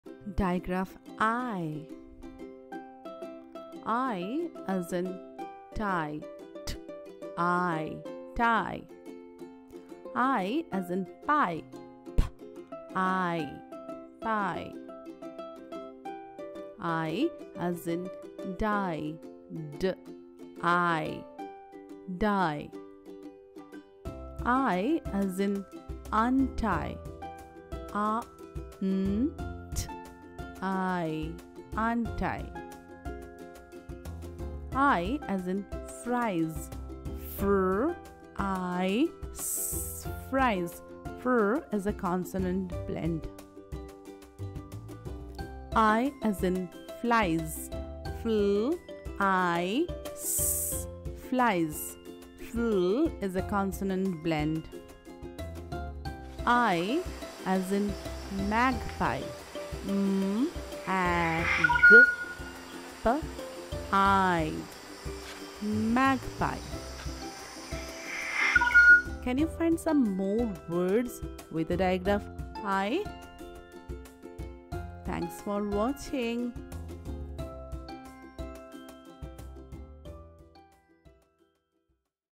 Digraph /ie/ | Phonics Lesson | Learn to Read with Phonics
Digraphs are two letters and a new sound.
Today we are learning the digraph /ie/
Digraph /ie/ makes a long vowel sound
/ie/ as in pie, die, tie, untie, flies, fries, terrified, magpie etc.
Follow us to learn everything about phonics
Digraph /ee/ makes a long vowel sound.